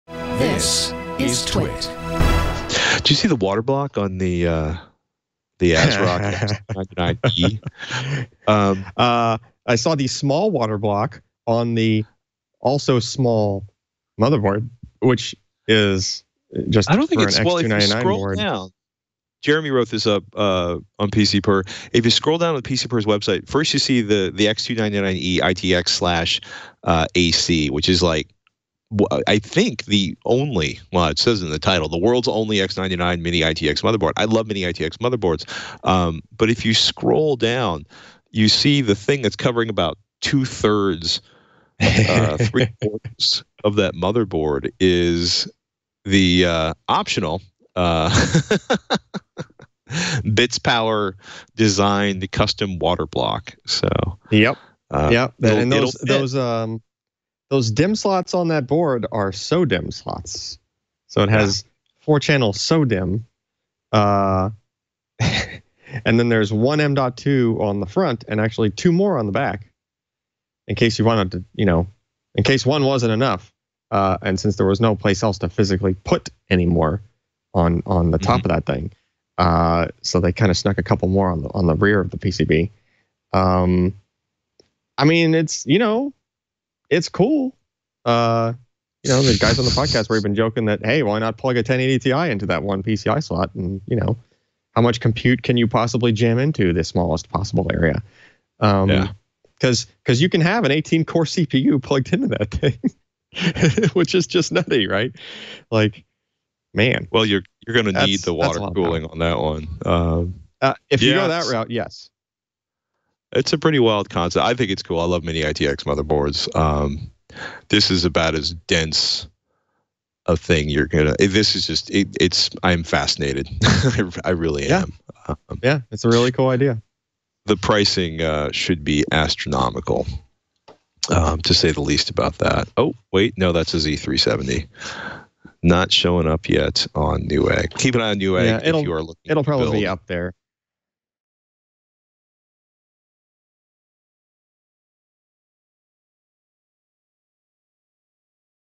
The Only X299 Mini-ITX Motherboard in the World
Patrick Norton and Allyn Malventano note ASRock's recent achievement of creating a high-end mini-ITX X299 motherboard, which works with a Bitspower custom waterblock for cooling. It has dual Intel Gigabit Lan and support for quad channel memory up to DDR4 4000MHz(OC).